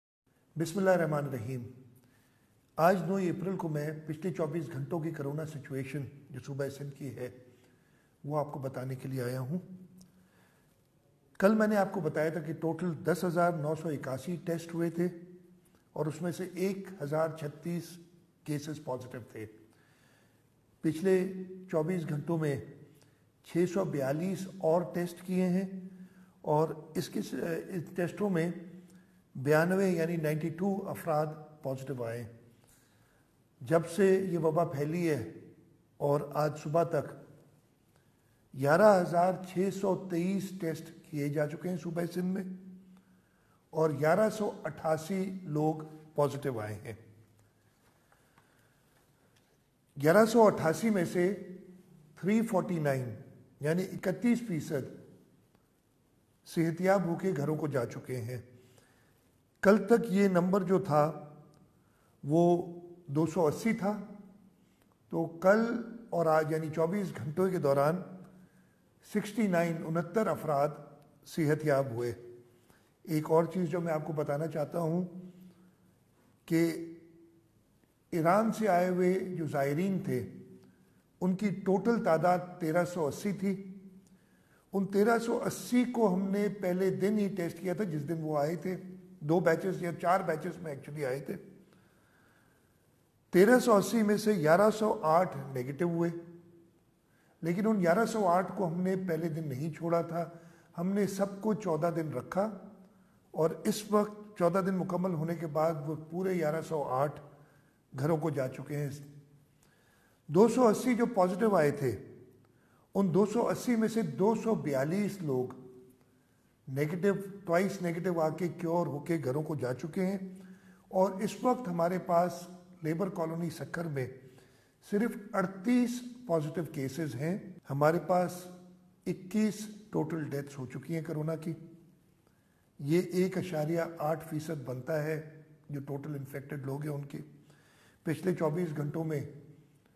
مجھے جس چیز کا ڈر تھا آج افسوس کے ساتھ وہ ہی ہوگیا، وزیراعلیٰ سندھ
* ایک ہی خاندان کے سات افراد میں کورونا ظاہر ہوا ہے، سید مراد علی شاہ
* ضلع سینٹرل کراچی کی کچی آبادی میں فیملی کا سربراہ باہر سے وائرس لیکر گھر گیا، وزیراعلیٰ سندھ